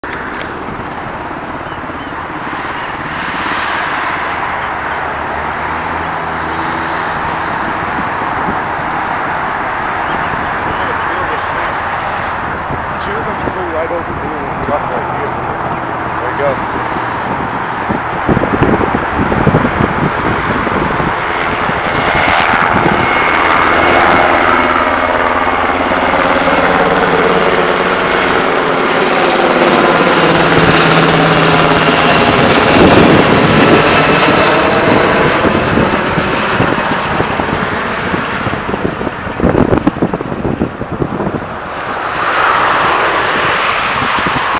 Sikorsky UH-60 Black Hawk Fly over
Shaky video I shouldn't have used a tripod